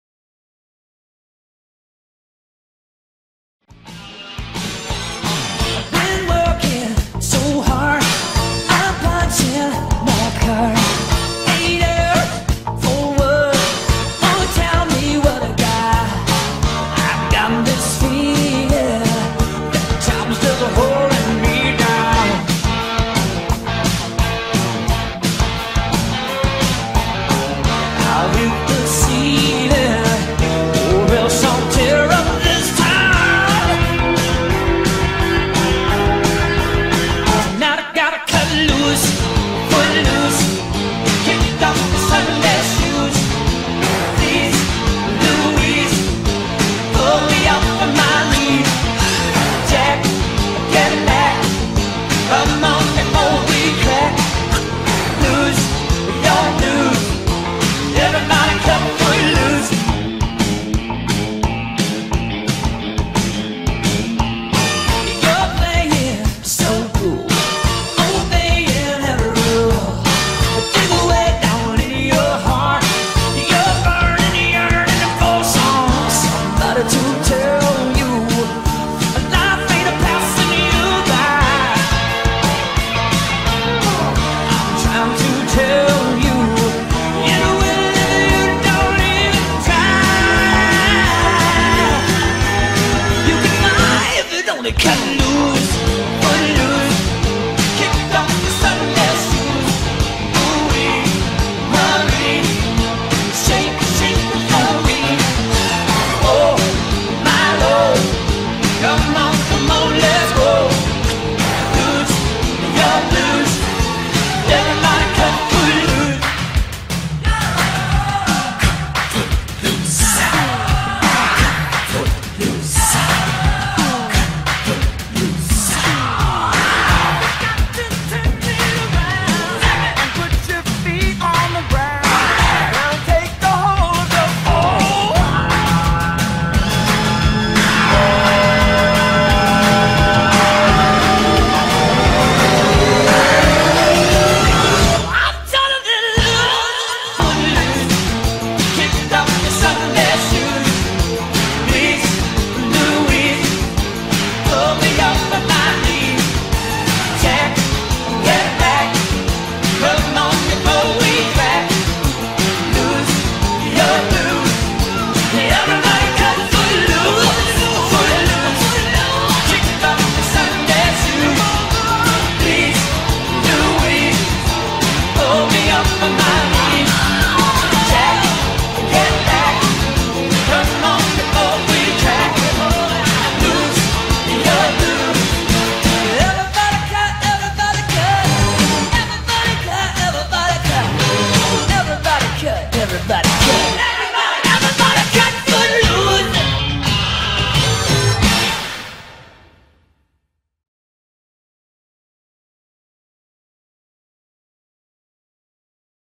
Felix Cleverest Cat - Entry for Magic
snow leopard Bengal - ex breeding queen - indoor cat - loves the play room designed for her - loving and very clever - too clever sometimes lol xt